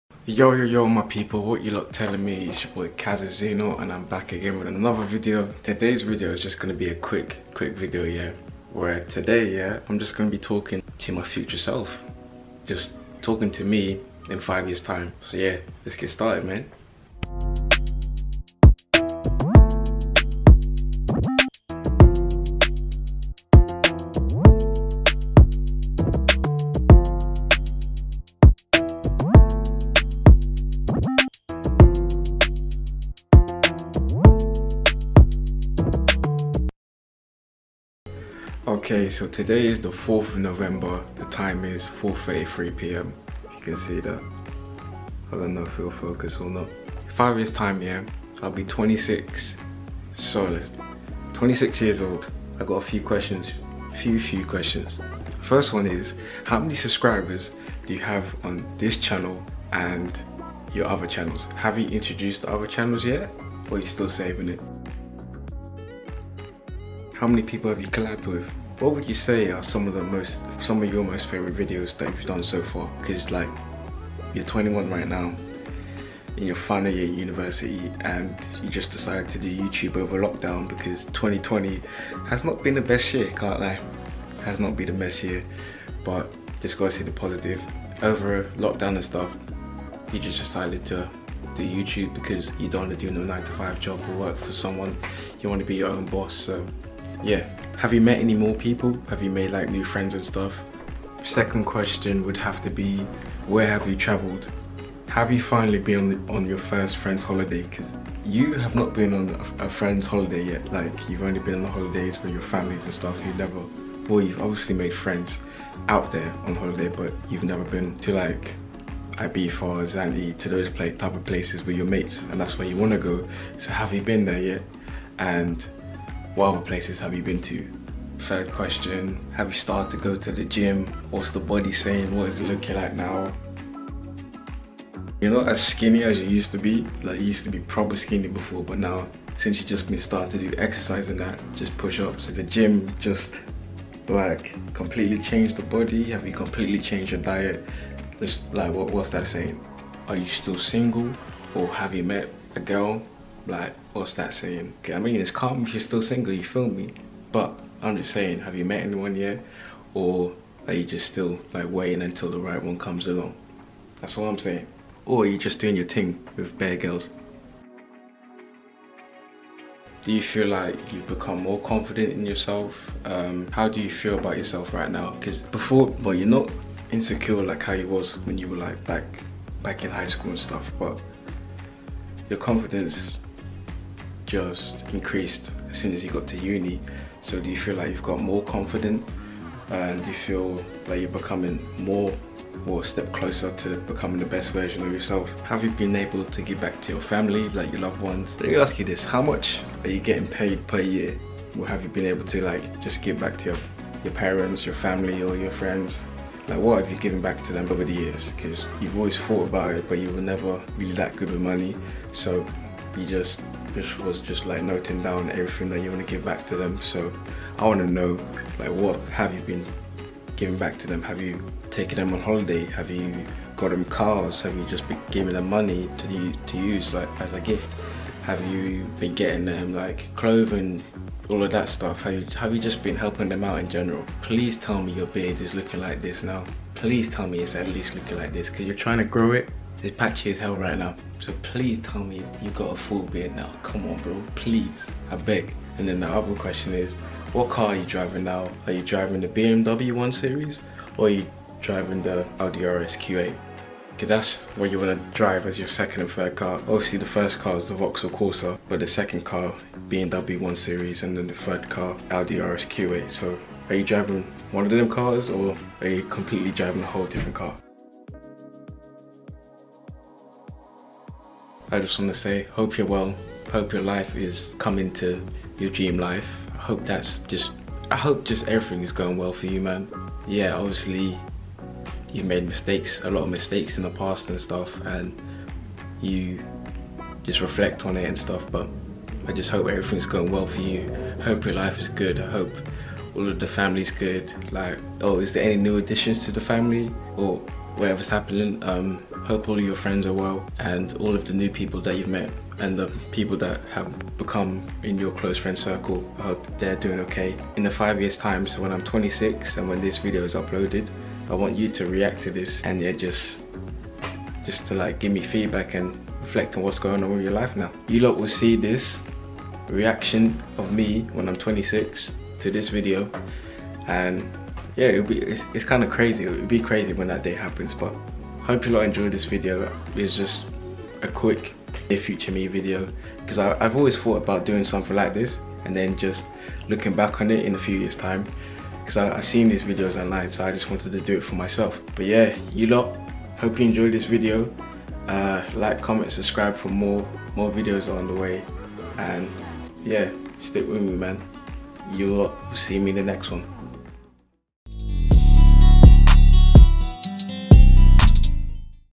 Dear Me in 5 Years
Today's video is something different. I decided to record a letter to my future self, well specifically my 26 year old self. I just want to see how my life has changed in 5 years time. Let's see what has changed.

Trying to get to at least 200 subscribers by the end of the year. I know we can do it. More videos are coming.